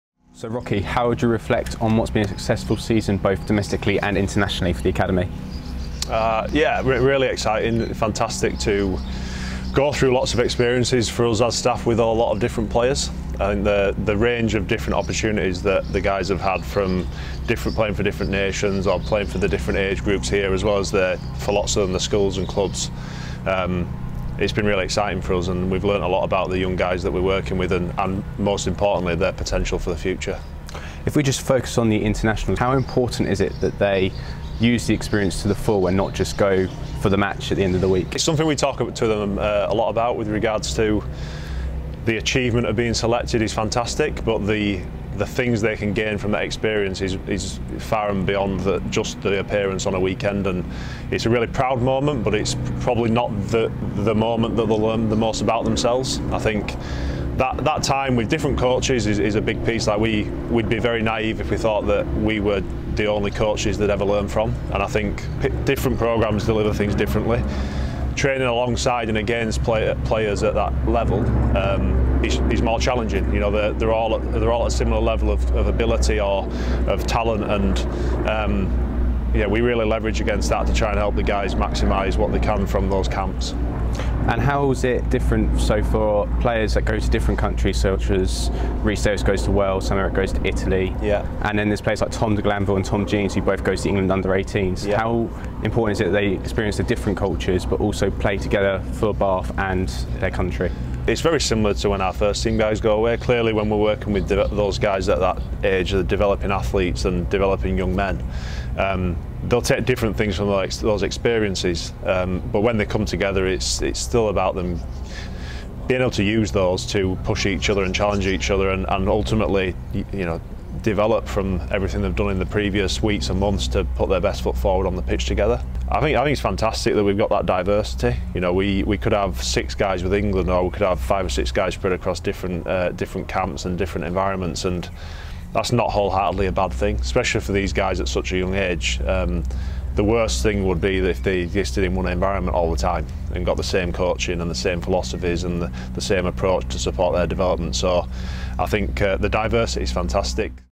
Academy review - Andy Rock
Academy Manager Andy Rock talks through a successful year for the Academy set-up domestically and on the international scene.